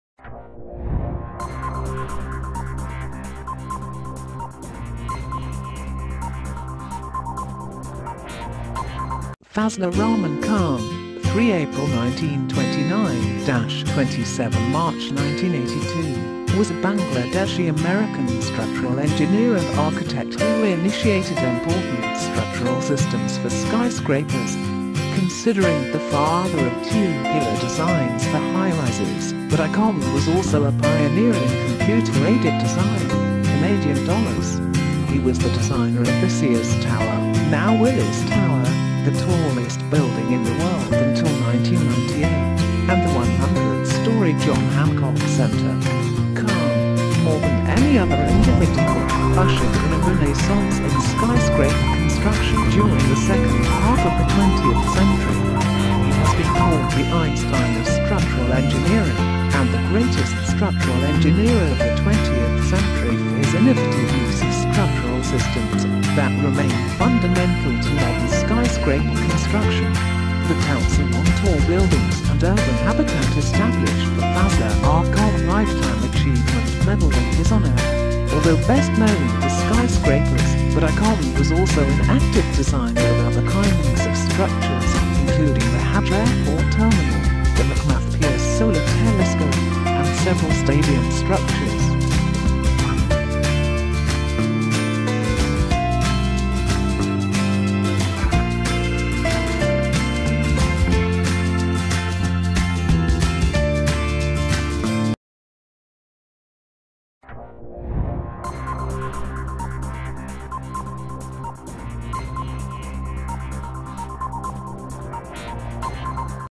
Fazlur Rahman Khan
Fazlur Rahman Khan (3 April 1929 – 27 March 1982) was a Bangladeshi-American fundamental fashioner and architect who began basic essential structures for elevated structures. Considered the "father of tubular layouts" for raised structures, Khan was in like manner a pioneer in PC upheld arrange (CAD). He was the fashioner of the Sears Tower (now Willis Tower), the tallest chipping away at the planet until 1998, and the 100-story John Hancock Center.

Khan, more than whatever other individual, presented a renaissance in skyscraper advancement in the midst of the second half of the twentieth century. He has been known as the "Einstein of essential building" and the "Best Structural Engineer of the twentieth Century" for his creative use of assistant systems that stay key to present day tall structure improvement. The Council on Tall Buildings and Urban Habitat set up the Fazlur R. Khan lifetime achievement embellishment in his regard.

Yet best known for elevated structures, Khan was also a dynamic maker of various sorts of structures, including the Hajj air terminal, the McMath–Pierce daylight based telescope, and a couple stadium structures